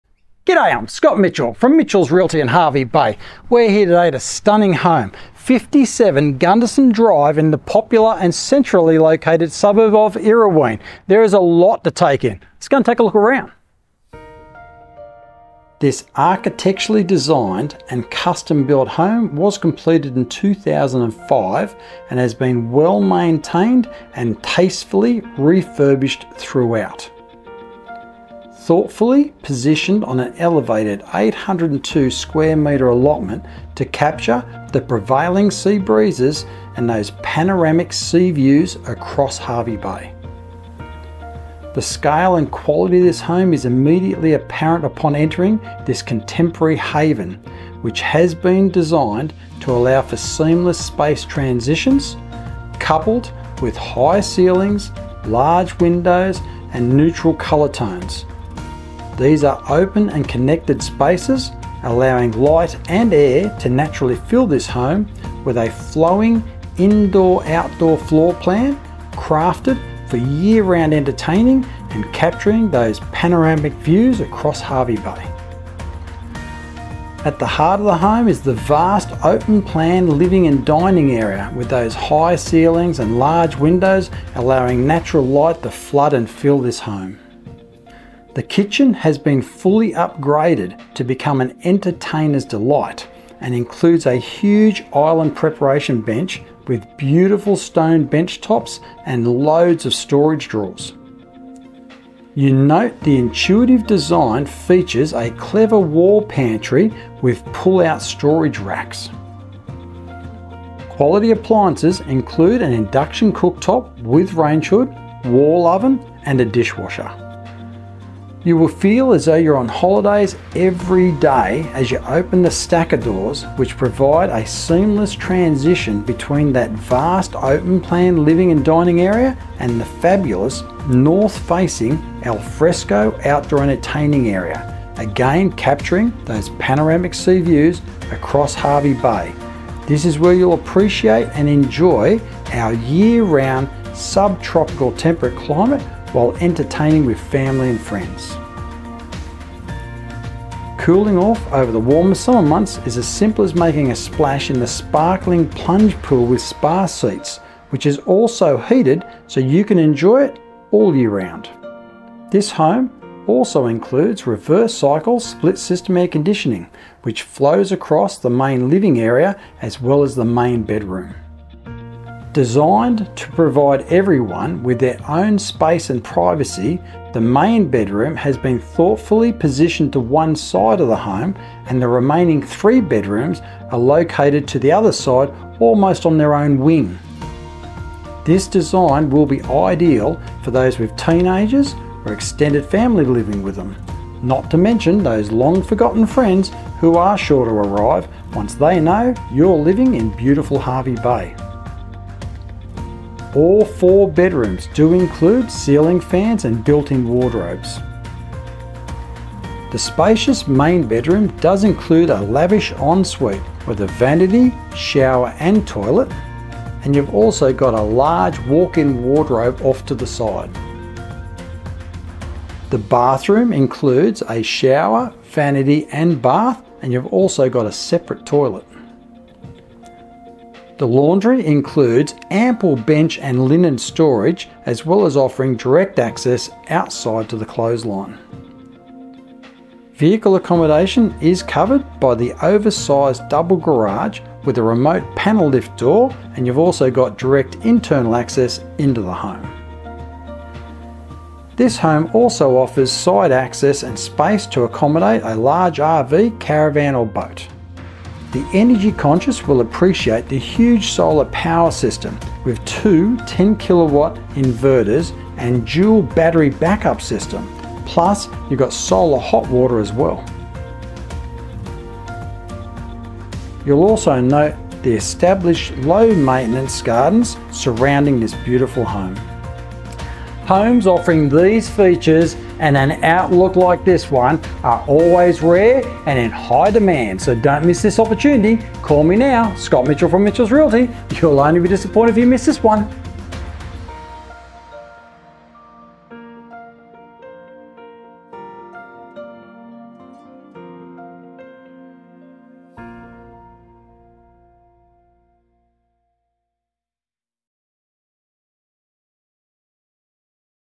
20230315 MITCHELLS 57 Gundesen Drive URRAWEEN
The Ultimate Sanctuary!

This architecturally designed and custom-built home was completed in 2005 and has been well-maintained and refurbished throughout and thoughtfully positioned on an elevated 802m2 allotment to capture the prevailing sea breezes and panoramic sea views across Hervey Bay.

The scale and quality of this home is immediately apparent upon entering this contemporary haven, designed to allow for seamless space transitions, coupled with high ceilings, large windows, and neutral colour tones. These open and connected spaces allow light and air to naturally fill this home with a flowing indoor/outdoor floor plan crafted for year-round entertaining and capturing the sea views across Hervey Bay.

57 Gundesen Dr Urraween features: -

• Elevated 802m2 allotment offering panoramic views across Hervey
• Stylish and tastefully refurbished modern contemporary home (built in 2005)
• Four spacious bedrooms with ceiling fans and built-in wardrobes
• The main bedroom includes a ceiling fan, split system air conditioning, a large walk-in wardrobe and a lavish ensuite
• The bathroom includes a shower vanity and bath, and there is a separate toilet
• A vast open-plan living and dining area with high ceilings and split-system air conditioning
• The new kitchen is an entertainer's delight and includes a huge island preparation bench with beautiful stone benchtops and loads of storage draws
• Appliances include an induction cooktop with rangehood, a wall oven and a dishwasher
• North-facing alfresco outdoor entertaining area – again capturing those panoramic sea views across Hervey Bay
• A heated plunge pool with spa seats
• The laundry includes ample bench & linen space and direct access outside to the clothesline
• A double garage with a remote panel lift door and direct internal access into the home
• Clear side access and space to accommodate large RVs, caravans and boats
• A huge solar power system with 2 x 10kw inverters and a dual battery backup system
• Established low-maintenance gardens

Homes of this calibre in this location do not become available often, and they are always highly sought after – so don't miss this opportunity!

Contact our team NOW to arrange your private inspection or a Facetime walk-through – you will only be disappointed if you miss this one …

NOTE: Property boundary lines are shown as approximate/indicative only in associated images - please refer to survey plans for complete details.